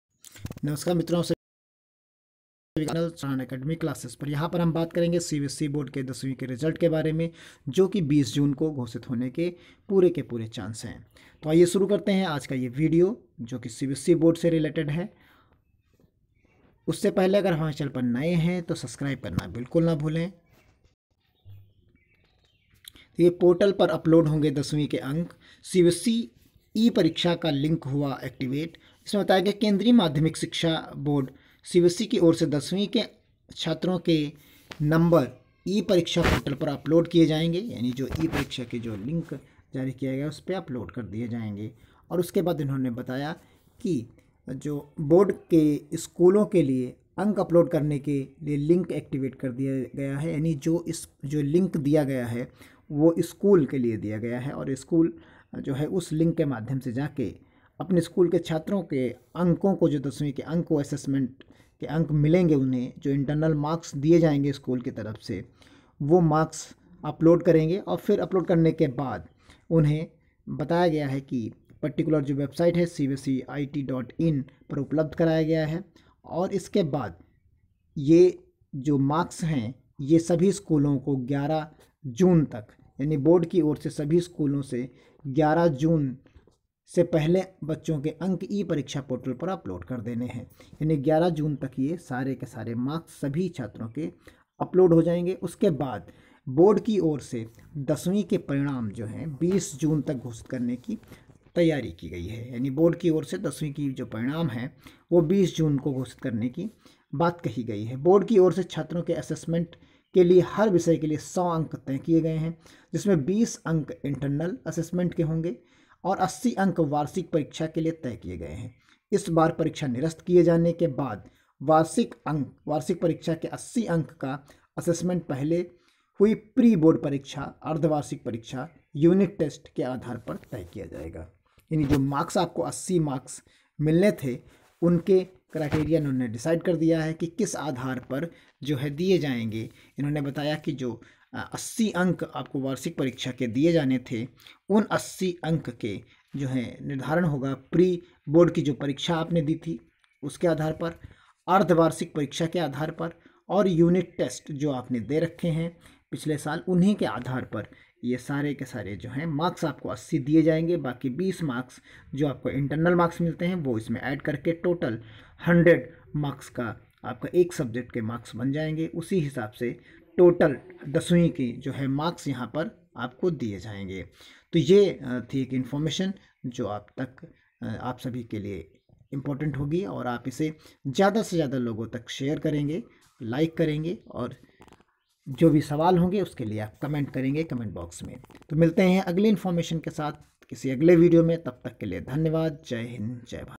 CBSE Board 10 वीं रिजल्ट 20 जून 2021 को होगा घोषित
CBSE Board 10 वीं रिजल्ट
 20 जून 2021 को होगा घोषित